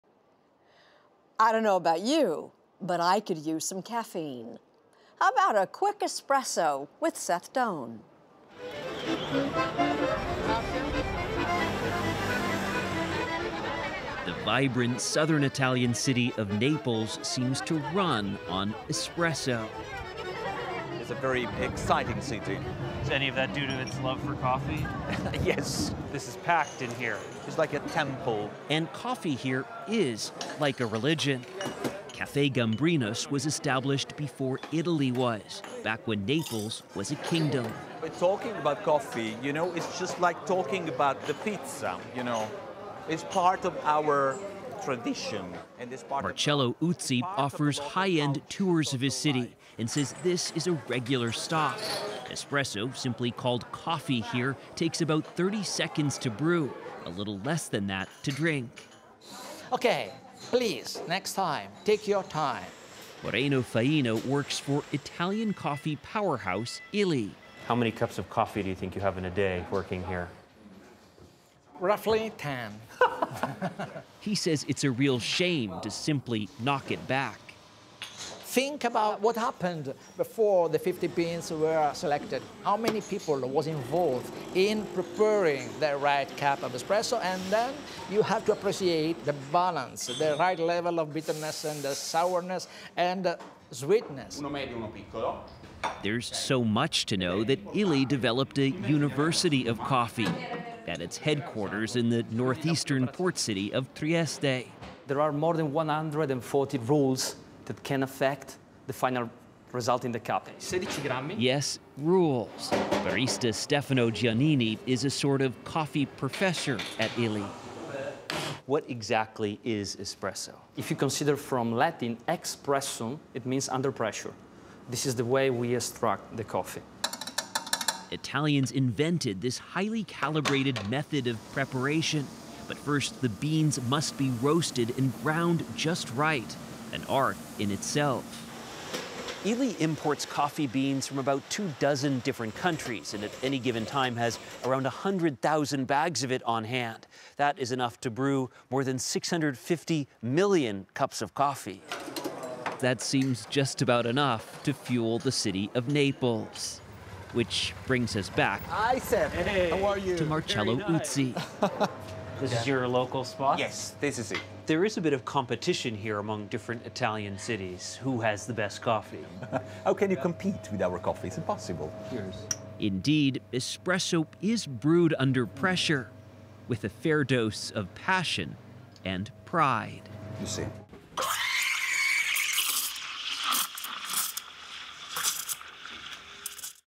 A perfect cup of espresso
The vibrant southern Italian city of Naples seems to run on espresso. Seth Doane explores the Neapolitan love of coffee with tour guide Marcello Uzzi at Caffè Gambrinus, and attends lessons at the Italian coffee powerhouse Illy's own University of Coffee in Trieste, where the highly-calibrated method of preparing the perfect cup of espresso is taught.